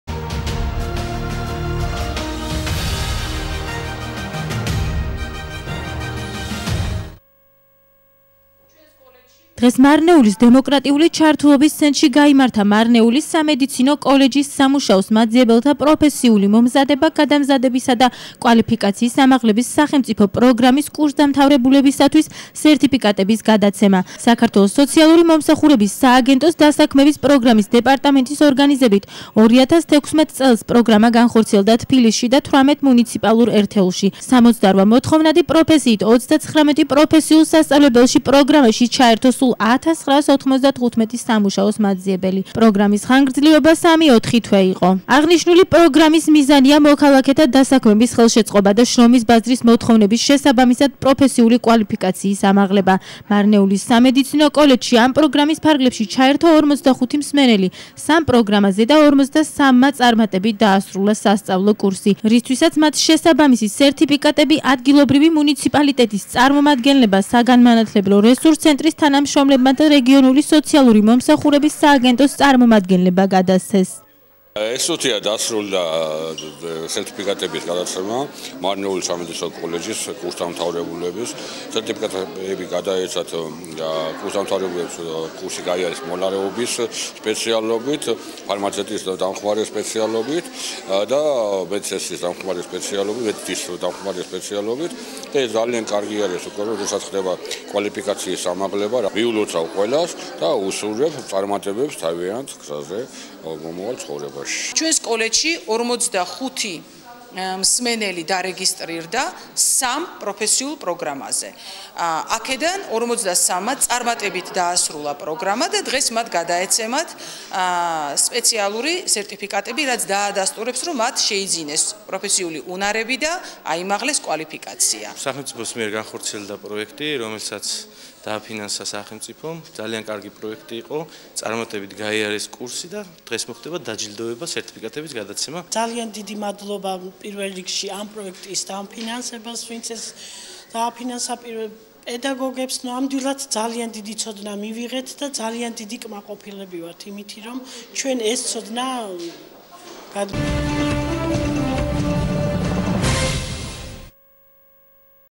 სამედიცინო კოლეჯის კურსამთავრებულებს სერტიფიკატები გადაეცათ (marneuli tv)
სამედიცინო კოლეჯის კურსამთავრებულებს სერტიფიკატები გადაეცათ
დღეს, მარნეულის დემოკრატიული ჩართულობის ცენტრში, „სამუშაოს მაძიებელთა პროფესიული მომზადება-გადამზადებისა და კვალიფიკაციის ამაღლების სახელმწიფო პროგრამის“ კურსდამთავრებულთათვის სერტიფიკატების გადაცემის ცერემონია გაიმართა.
ღონისძიებაზე მიწვეულნი იყვნენ მარნეულის მუნიციპალიტეტის გამგებელი მერაბ თოფჩიშვილი, მოადგილეები, მარნეულის სოციალური სააგენტოს ხელმძღვანელი პირები და რესურს–ცენტრის წარმომადგენლები.
აღსანიშნავია, რომ საქართველოს სოციალური მომსახურების სააგენტოს დასაქმების პროგრამების დეპარტამენტის ორგანიზებით 2016 წელს, პროგრამა განხორციელდა თბილისში და 18 მუნიციპალურ ერთეულში. პროგრამაში სულ 1995 სამუშაოს მაძიებელი ჩაერთო.
აღნიშნული პროგრამის მიზანია მოქალაქეთა დასაქმების ხელშეწყობა და შრომის ბაზრის მოთხოვნების შესაბამისად პროფესიული კვალიფიკაციის ამაღლება.
მარნეულის სამედიცინო კოლეჯში ამ პროგრამის ფარგლებში ჩაერთო 45 მსმენელი სამ პროგრამაზე და 43-მა მსმენელმა წარმატებით დაასრულა სასწავლო კურსი, რისთვისაც მათ შესაბამისი სერტიფიკატები საზეიმო ვითარებაში გადასცეს ღონისძიებაზე მიწვეულმა სტუმრებმა.